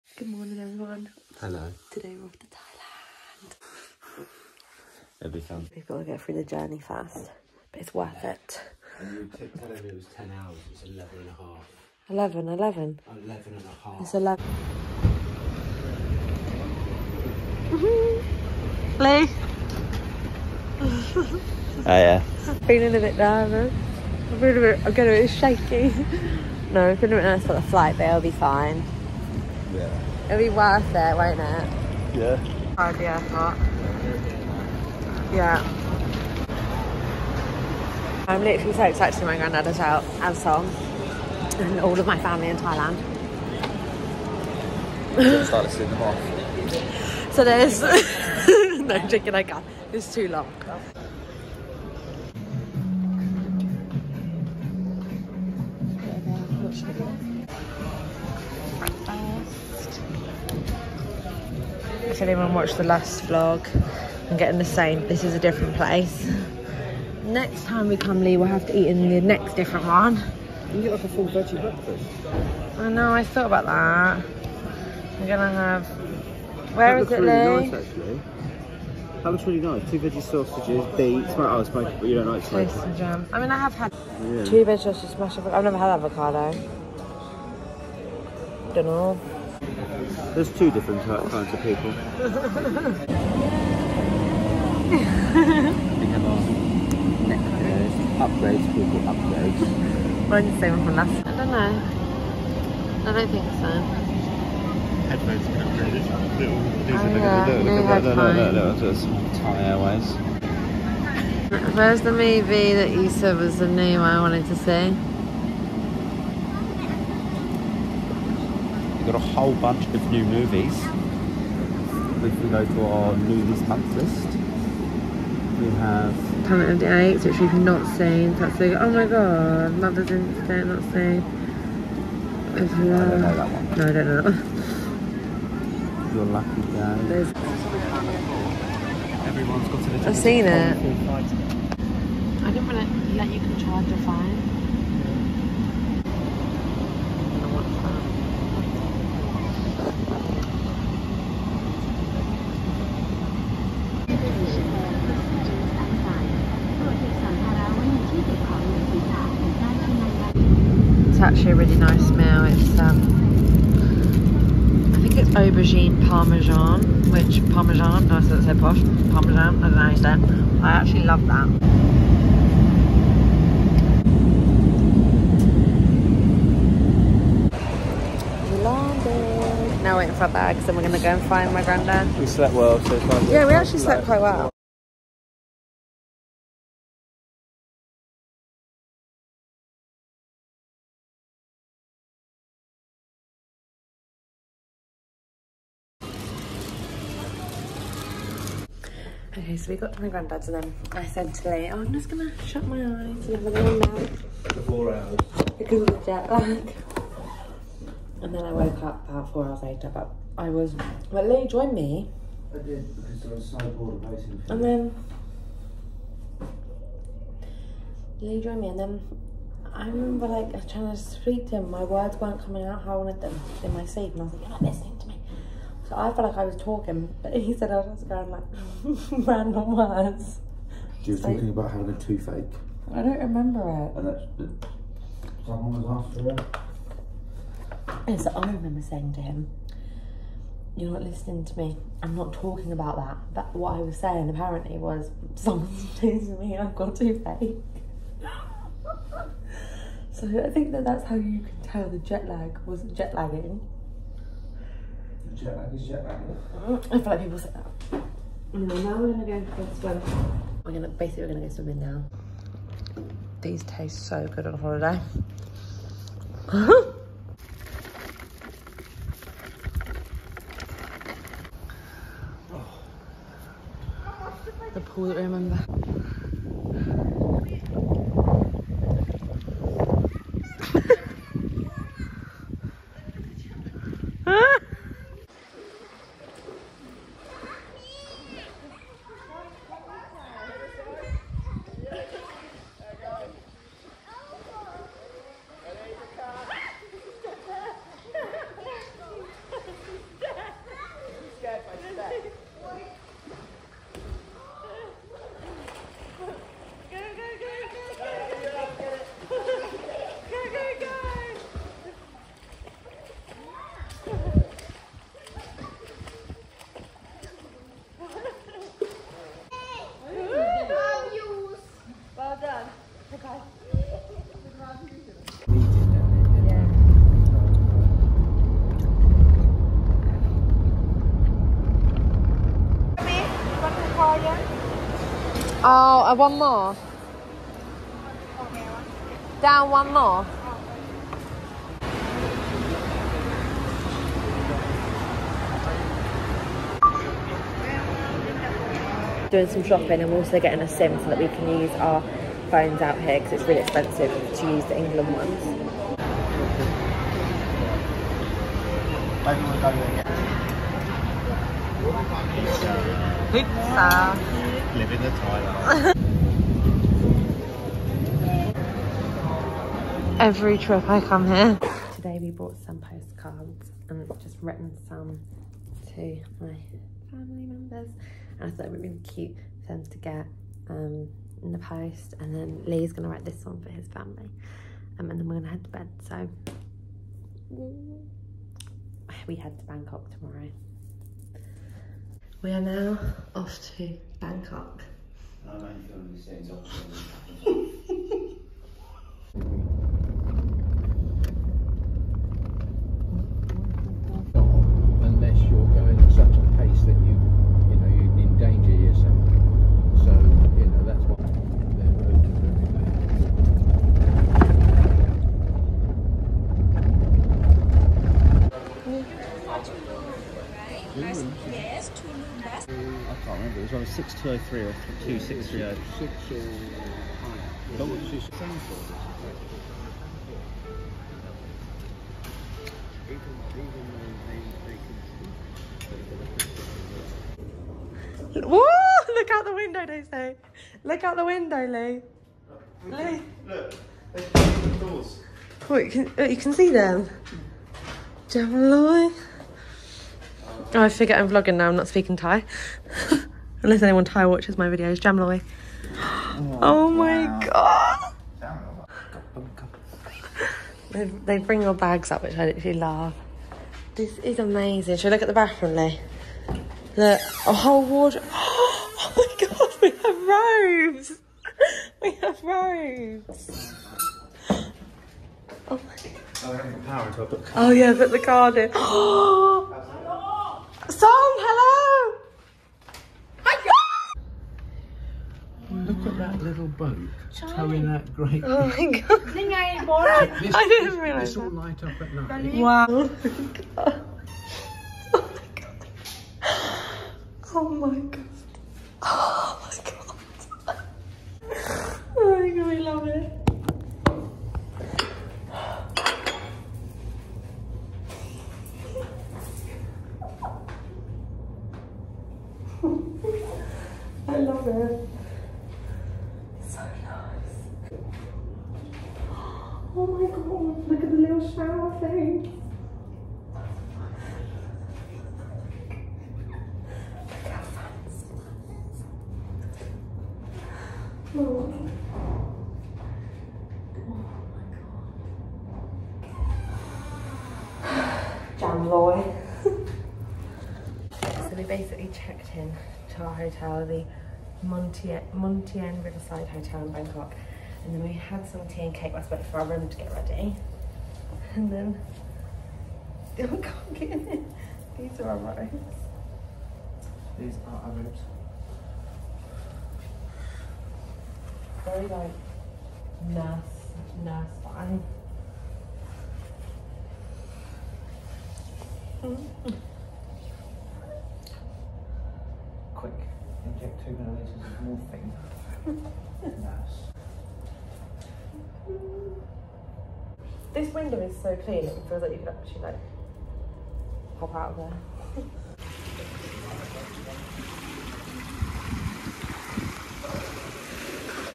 Flying Thai airways, arriving in Thailand and hotel room tour ✨🇹🇭
Today’s video is a vlog of us arriving in Thailand and exploring our Bangkok hotel ❤️

Depop: @abscadman